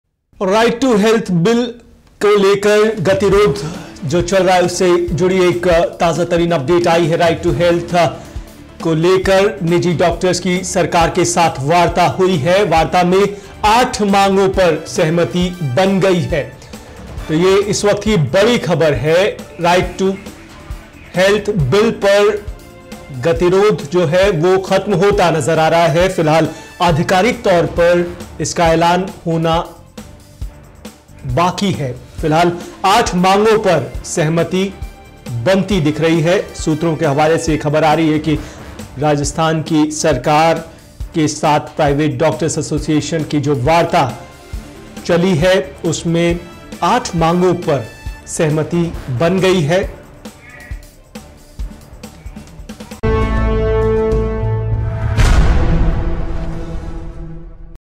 Breaking : RTH बिल को लेकर निजी डॉक्टर्स की हड़ताल ख़त्म, सीएस के साथ वार्ता में बनी सहमति
राइट टू हेल्थ बिल को लेकर बड़ी ख़बर
सीएस के साथ वार्ता में बनी सहमति
निजी डॉक्टर्स की हड़ताल ख़त्म
सरकारी सहायता नहीं लेने वाले से निजी अस्पताल RTH से बाहर
प्राइवेट मेडिकल कॉलेज पर RTH होगा लागू
पीपीपी मोड पर बने अस्पतालों पर लागू होगा RTH
सरकारी सहायता प्राप्त अस्पतालों पर RTH होगा लागू
ट्रस्ट वाले निजी अस्पतालों पर भी RTH लागू होगा